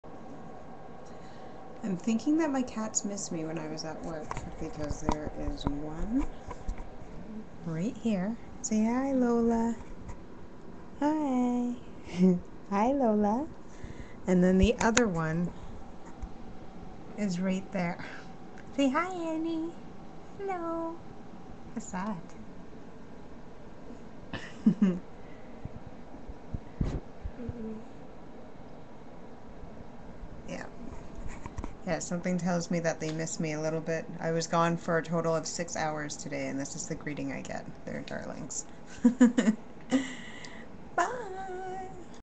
Cute adorable cats
I just got home from work and sat on my computer. This is what happens when I get home from work and sit at my computer. My Needy key-keys love me. :)